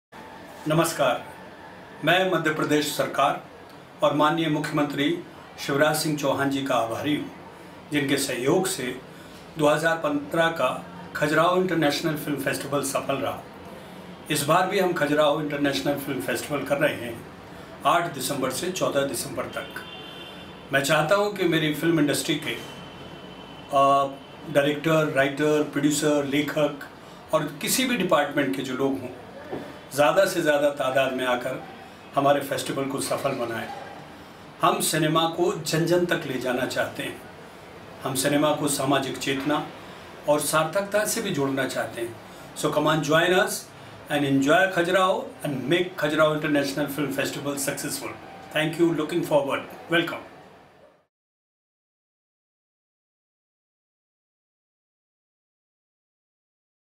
KIFF 2016 - Welcome address by Shri Raja Bundela
We are proud to announce the 2nd Khajuraho International Film Festival to be held from 8 - 14 December, 2016 at Khajuraho, Madhya Pradesh organised by Prayaas Productions Pvt. Ltd, Mumbai in association with the Government of Madhya Pradesh. Here is a welcome message by Shri Raja Bundela Ji, the Founder of KIFF.